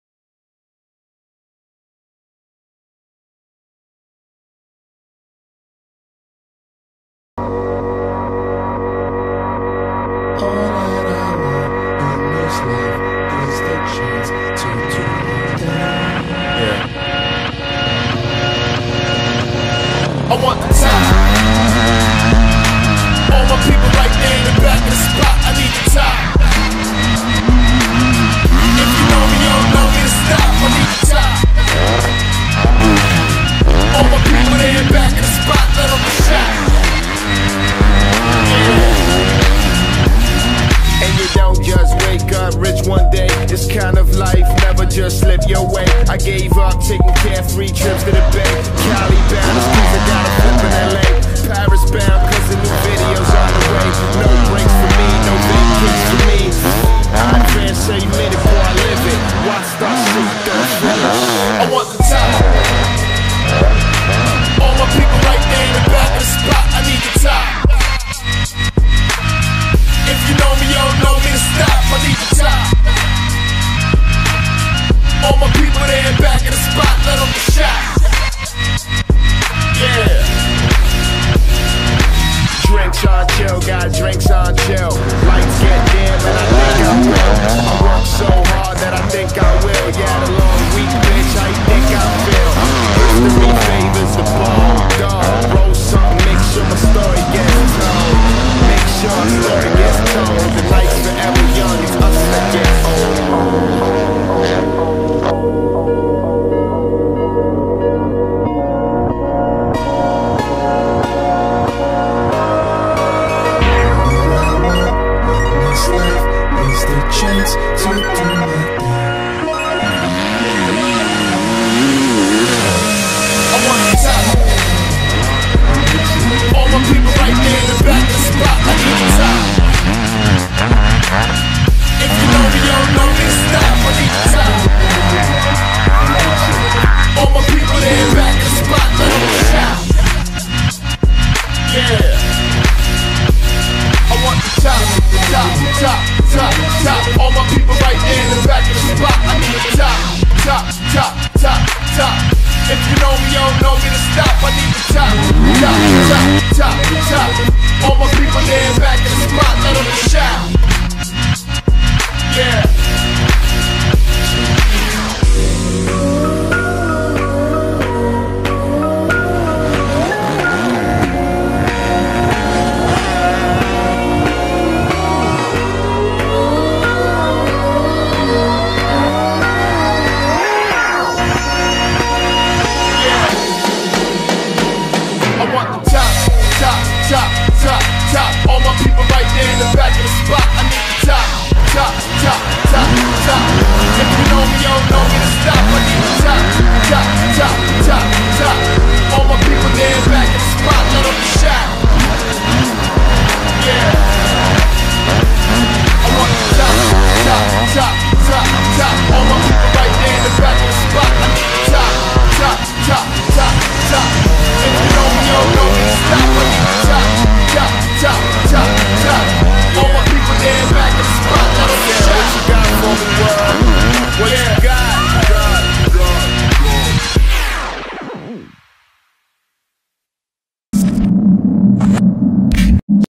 Chasing the Sand ft Jeffrey Herlings
Watch Jeffrey Herlings riding a sand track